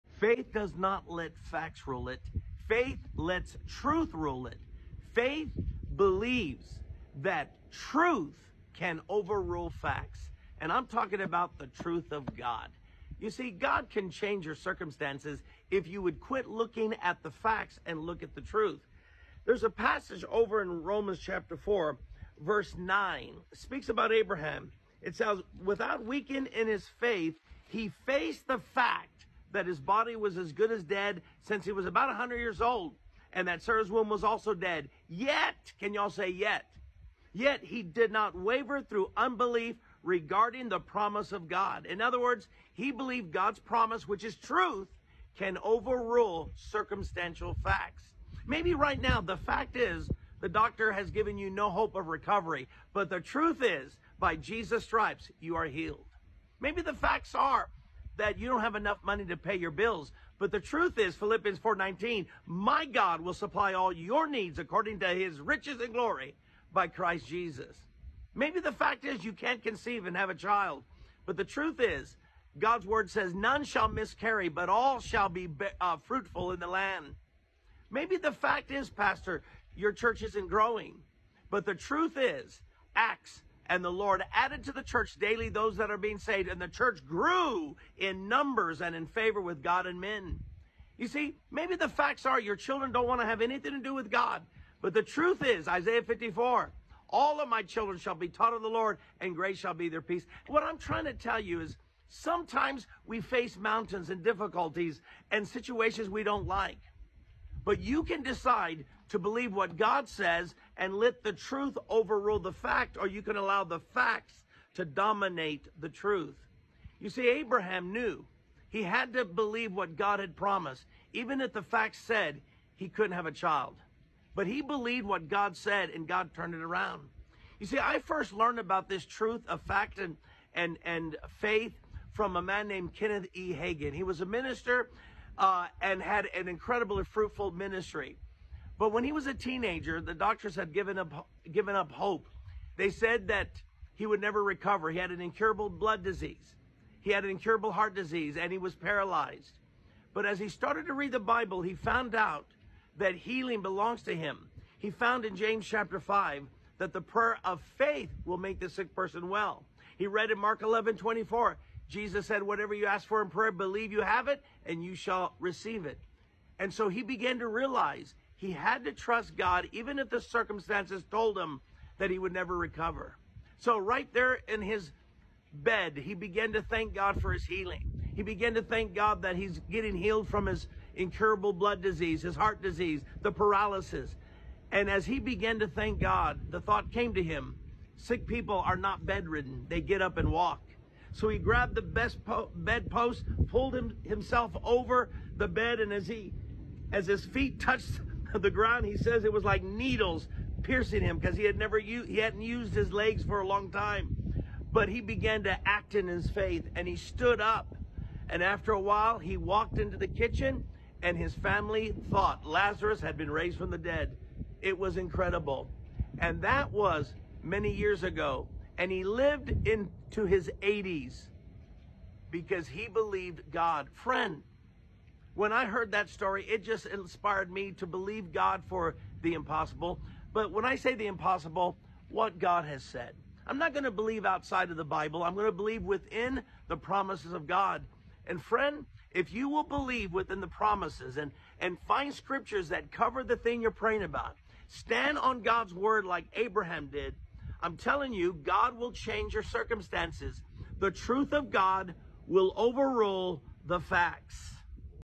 God Can Change Your Circumstances
Follow Abraham’s example who face the facts, yet trusted in God’s promise.